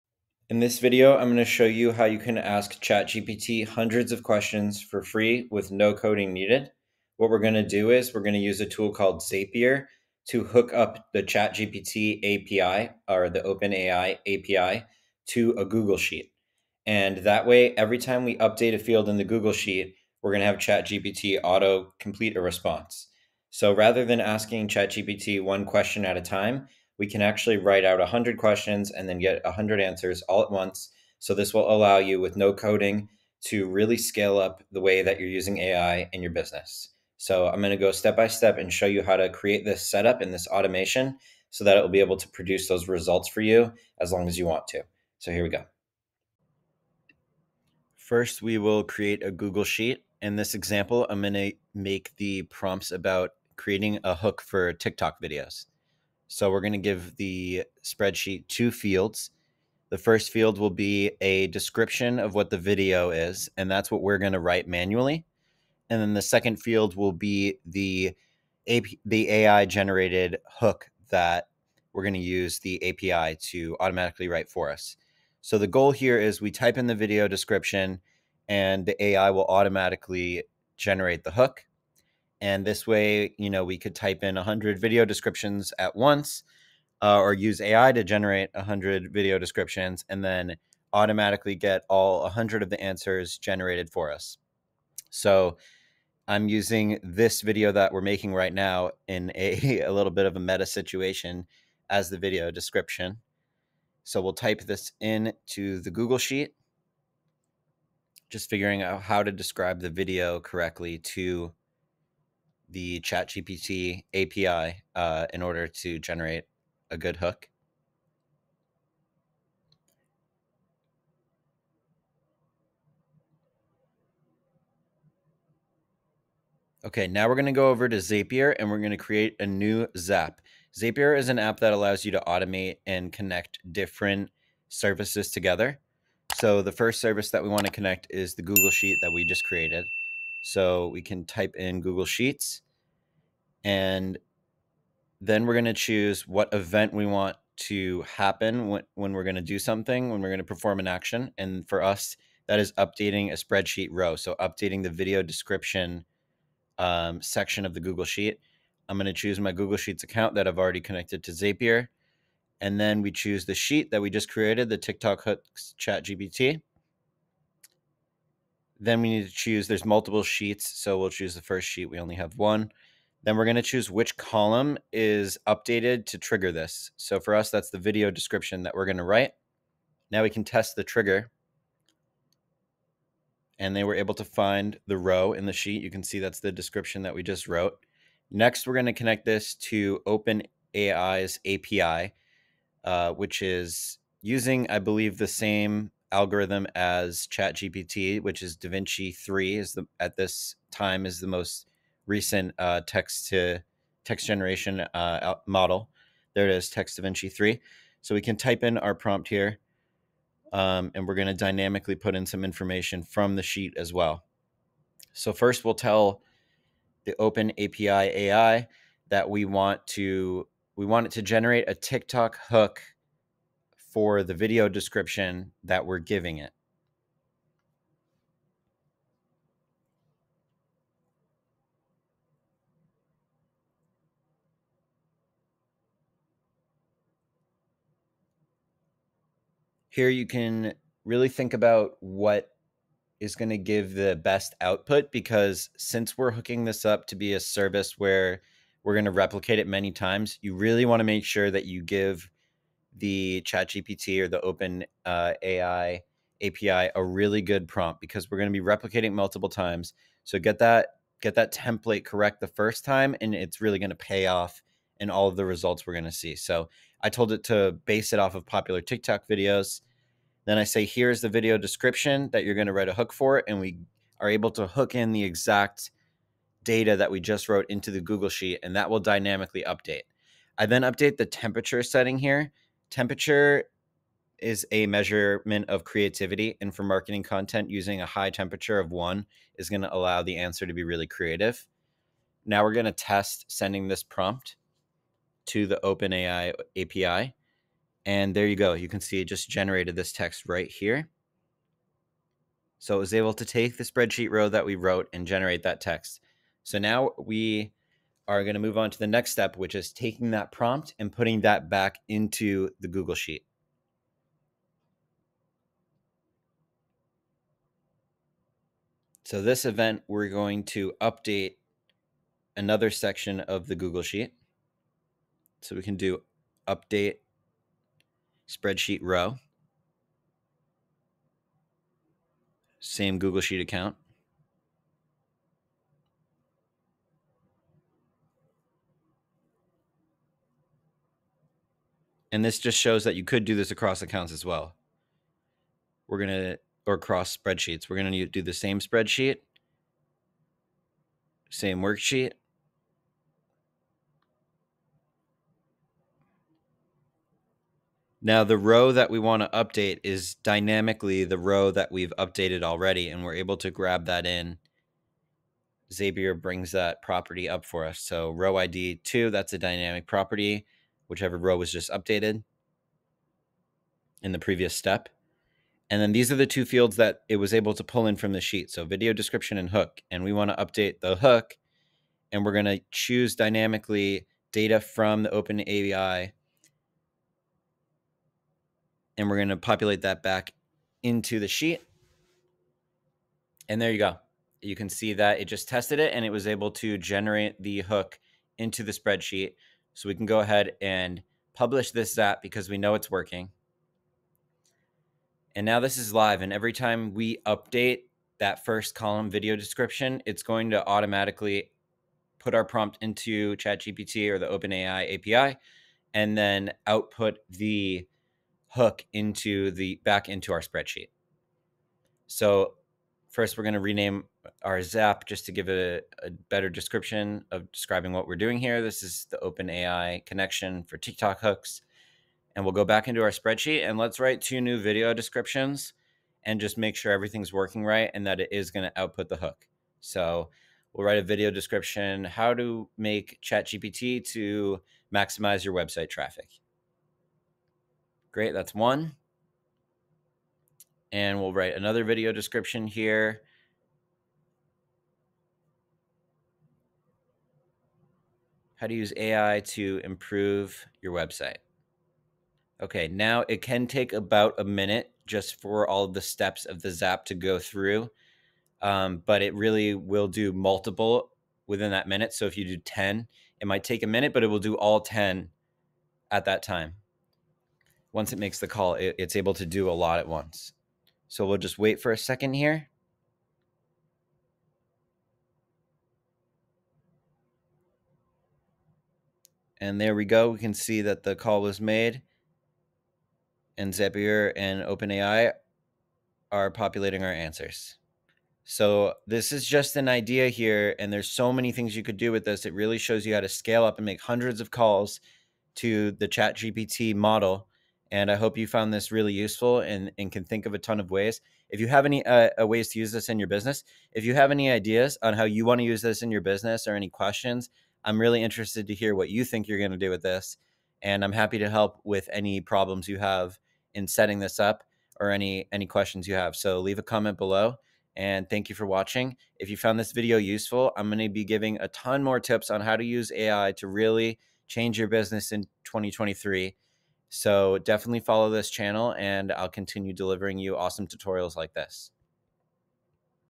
ChatGPT Guide | Get 100+ Answers at Once | OpenAI API Zapier Tutorial
How to use OpenAI API and Zapier to automate producing large scale ChatGPT answers.  Automate asking questions to chatGPT and scale your AI output with this strategy.

Why generate 1 AI response at a time in chat when you can do 100+ at once?